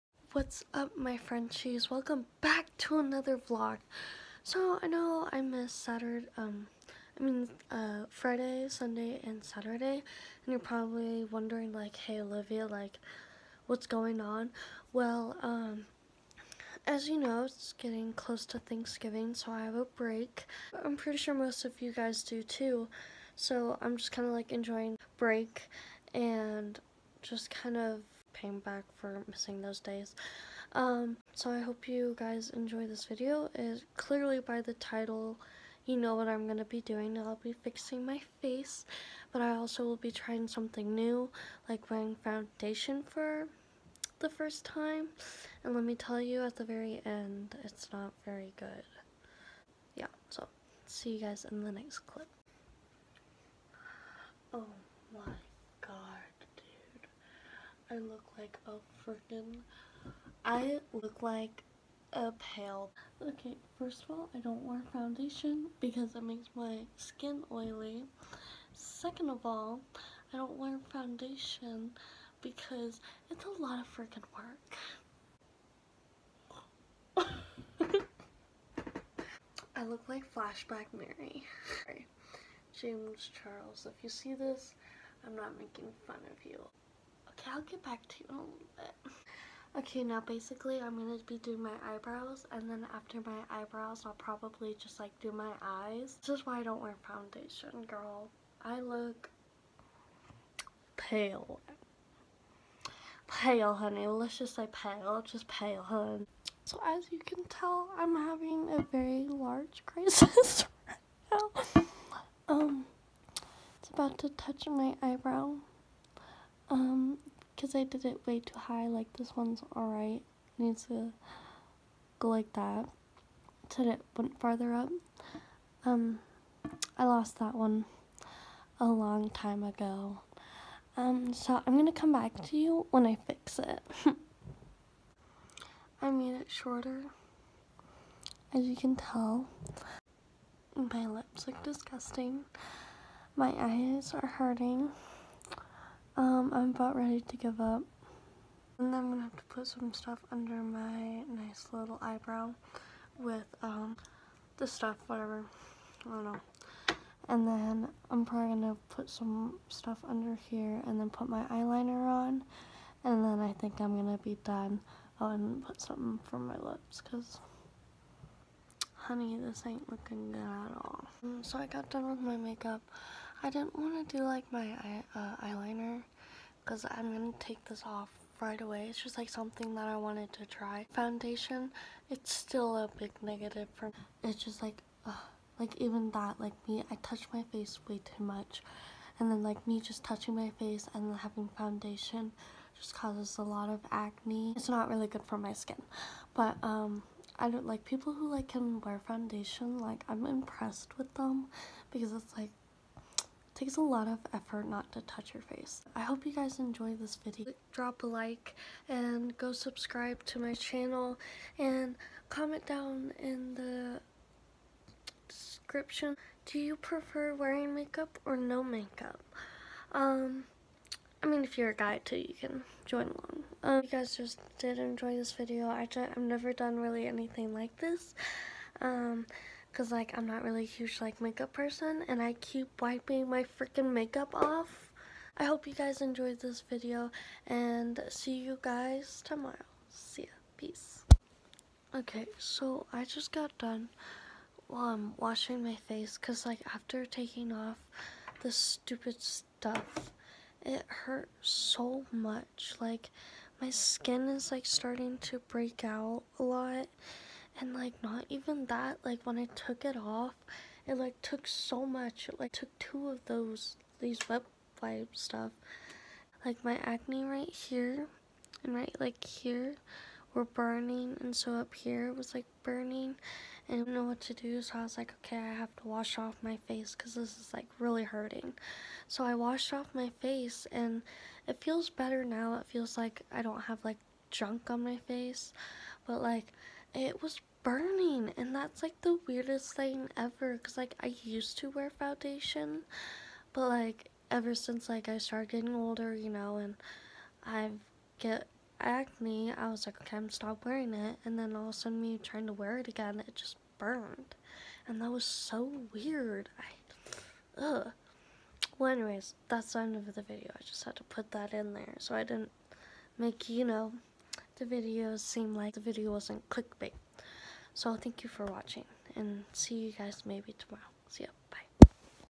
TRYING SOMETHING NEW WITH MY MAKEUP! * face started to burn *
Sc: Oliviamfrench
I’ve had my makeup burn my face before it was so weird!